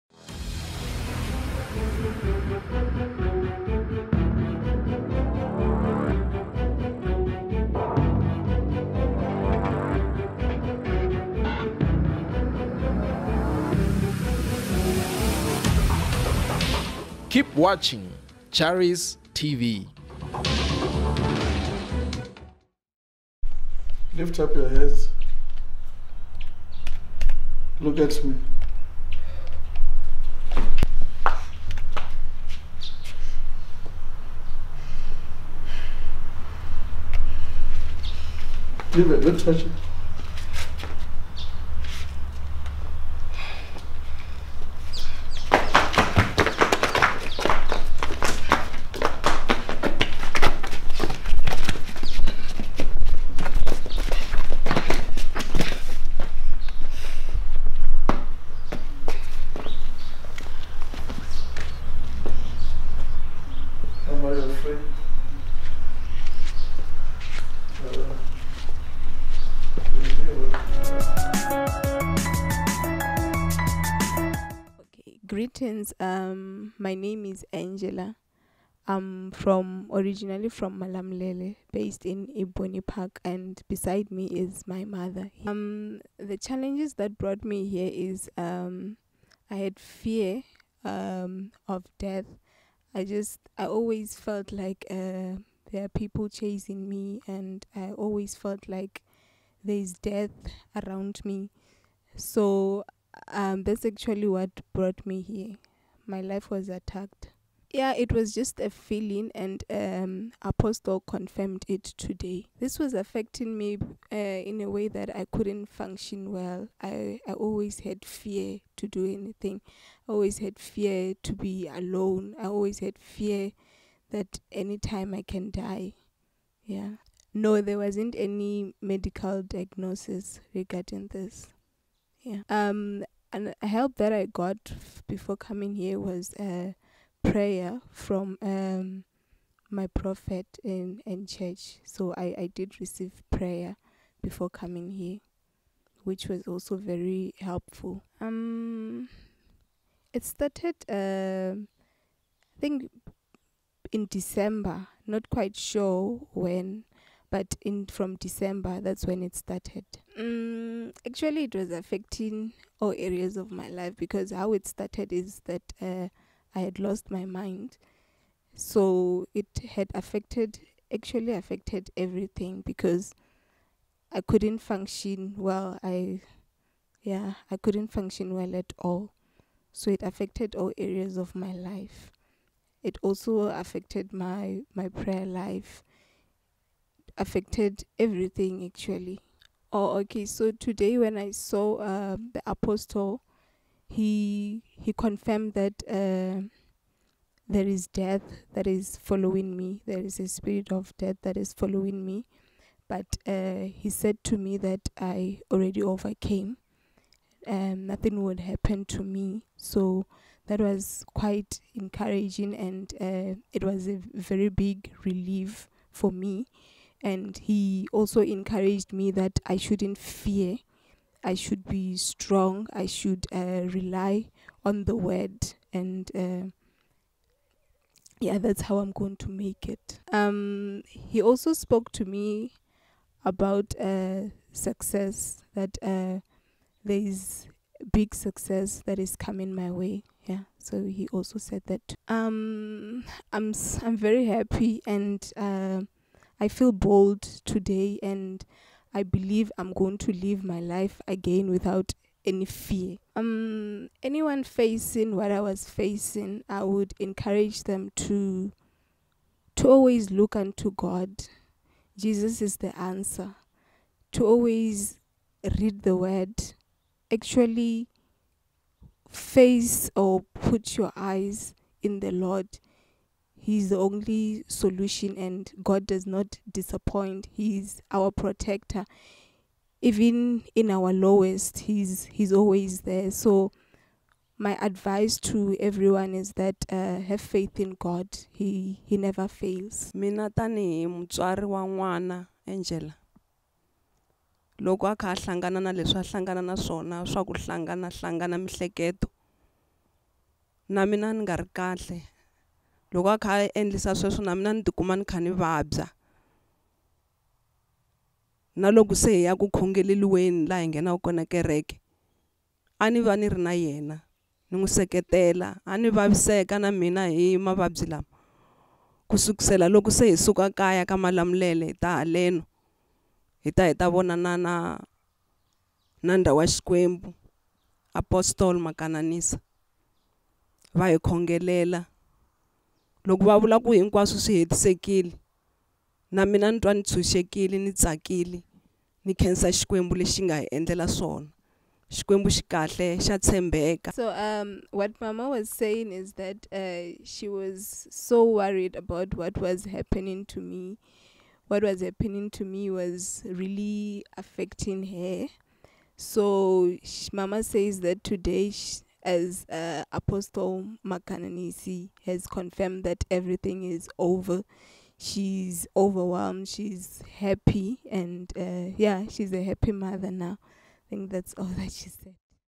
❗ CHARIS VISITOR'S EXPERIENCE ❗ One on One Session – DELIVERANCE FROM FEAR OF DEATH
This woman came to be delivered from the fear of death, which was confirmed by Apostle BJ Makananisa during her one on one session with the Man of God. She was urged to rely on the Word of God, to live her life without fear and she was further assured that success was coming her.